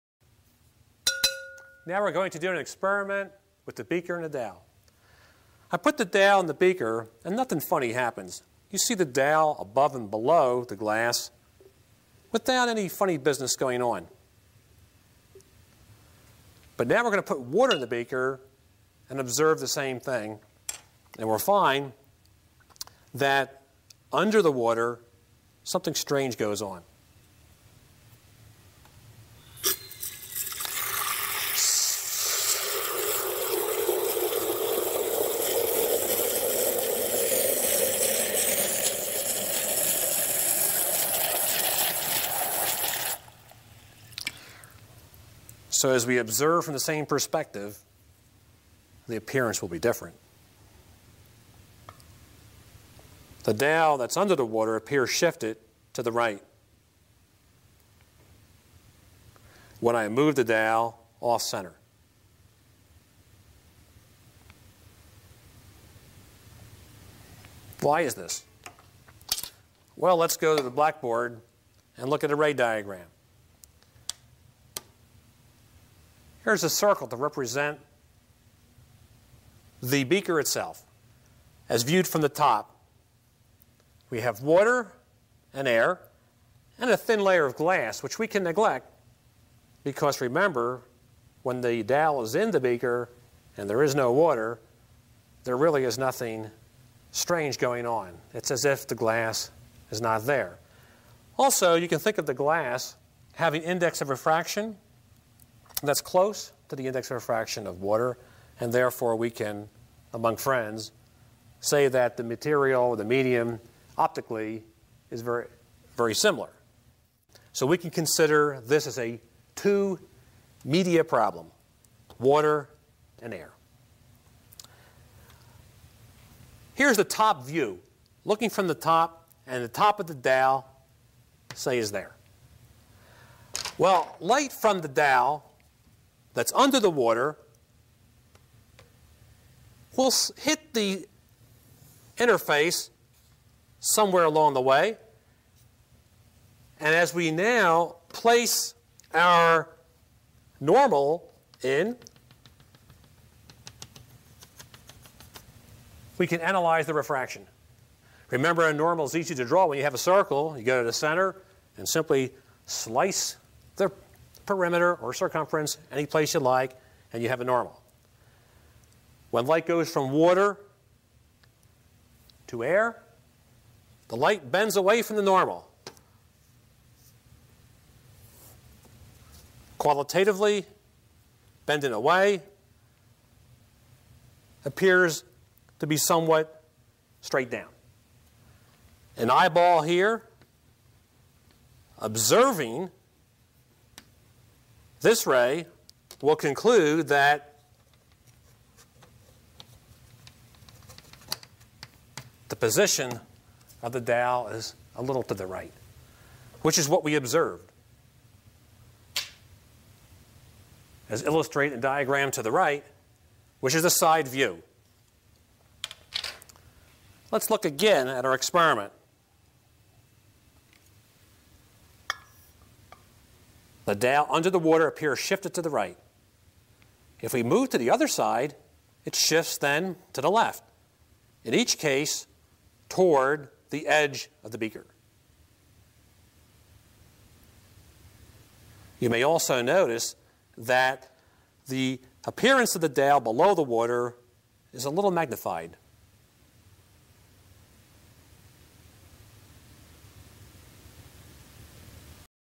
Refraction of Rod in Water
The refraction of a rod in water is analyzed with a ray diagram.